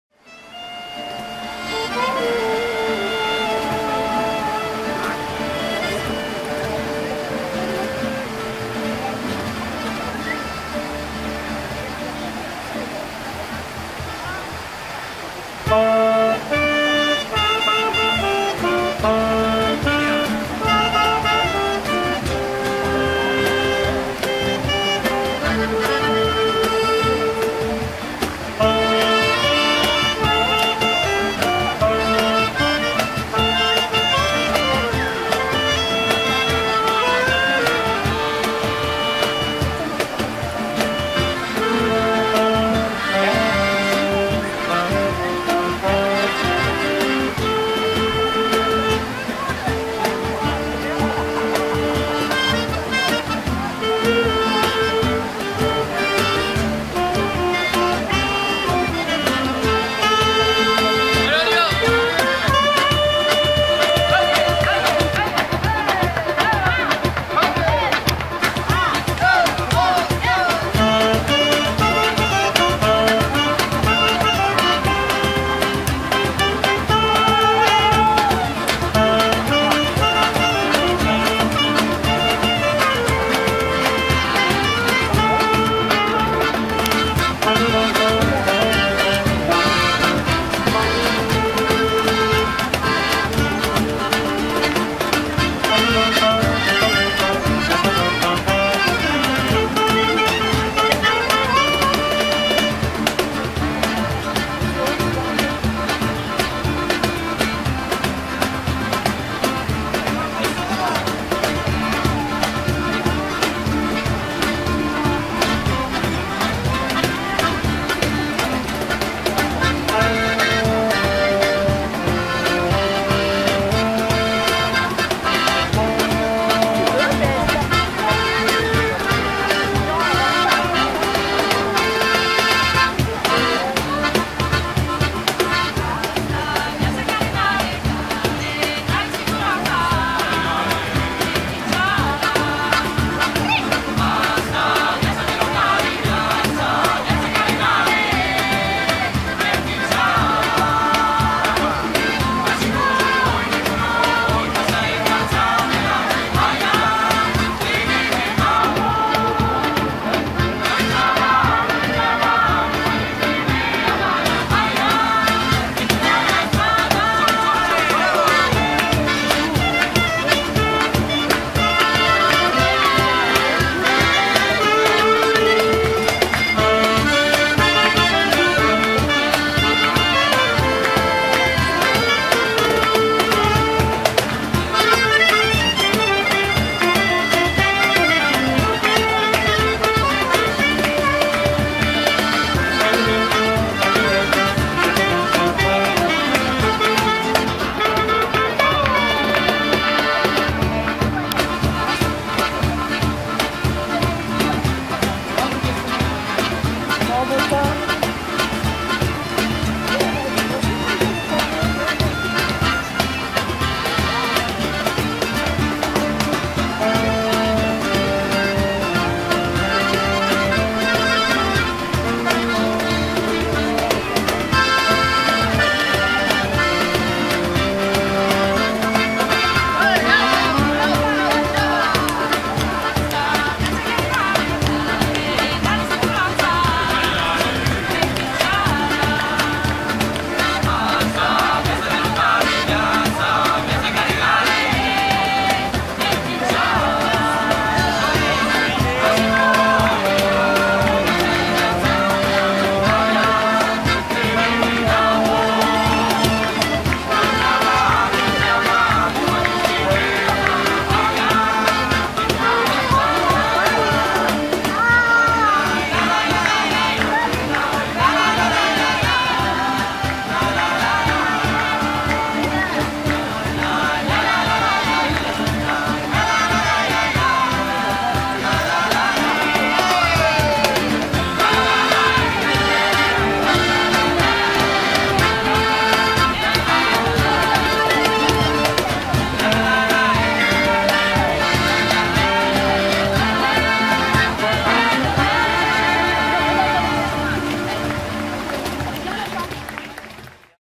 Pont en Royans : passage musical
(sélectionner qualité HD dans paramètres !!!)
20 juillet 2016 : un petit concert  plein air bien sympa
Si quelqu'un connaît le nom de ce groupe, merci de me le faire savoir